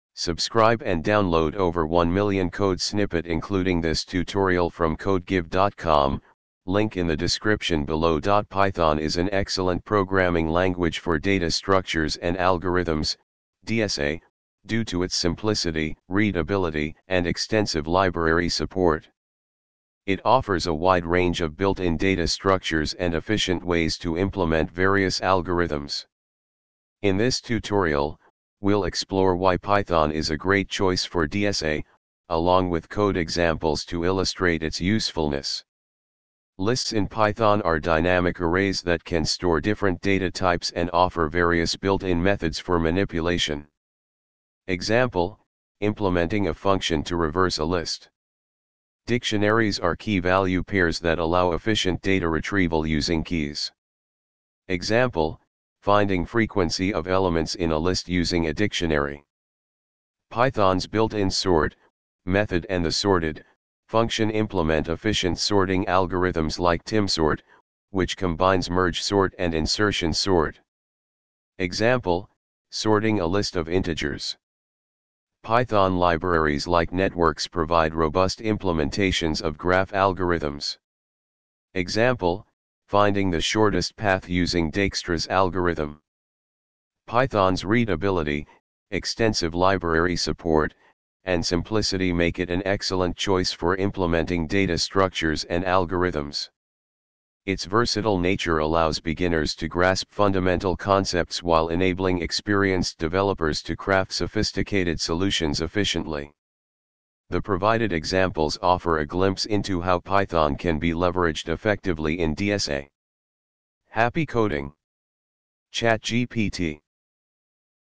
is python good for dsa
Python is an excellent programming language for Data Structures and Algorithms (DSA) due to its simplicity, readability, and extensive library support. It offers a wide range of built-in data structures and efficient ways to implement various algorithms. In this tutorial, we'll explore why Python is a great choice for DSA, along with code examples to illustrate its usefulness.
Lists in Python are dynamic arrays that can store different data types and offer various built-in methods for manipulation.
Example - Implementing a function to reverse a list:
Dictionaries are key-value pairs that allow efficient data retrieval using keys.
Example - Finding frequency of elements in a list using a dictionary:
Python's built-in sort() method and the sorted() function implement efficient sorting algorithms like Timsort, which combines merge sort and insertion sort.
Example - Sorting a list of integers:
Python libraries like networkx provide robust implementations of graph algorithms.
Example - Finding the shortest path using Dijkstra's algorithm:
Python's readability, extensive library support, and simplicity make it an excellent choice for implementing Data Structures and Algorithms. Its versatile nature allows beginners to grasp fundamental concepts while enabling experienced developers to craft sophisticated solutions efficiently. The provided examples offer a glimpse into how Python can be leveraged effectively in DSA. Happy coding!
ChatGPT